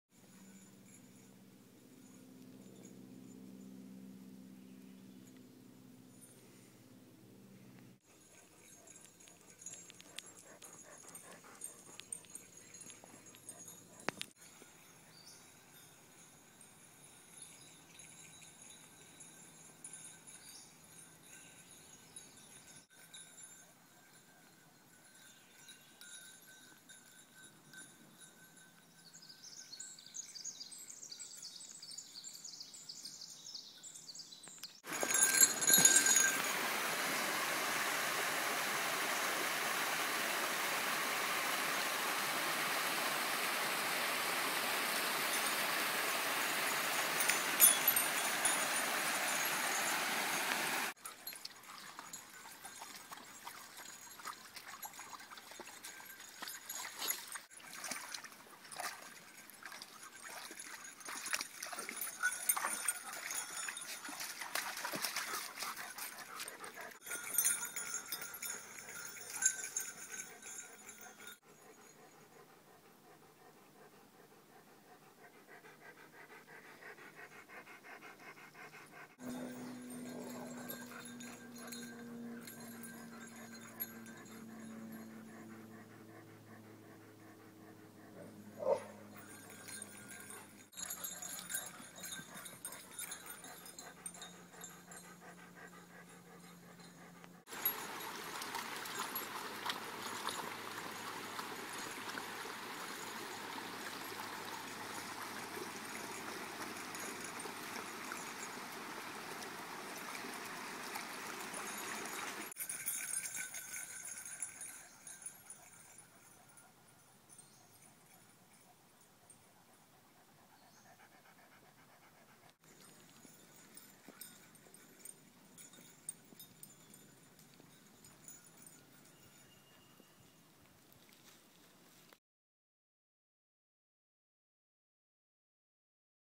5 Dates with Sunshine
Wednesday July 16, 2014 Adventure dog walk with Bailey the Golden Retriever, Nila the Desi Dog, Stella the Border Collie, with Beckem and Logan the Goldendoodles.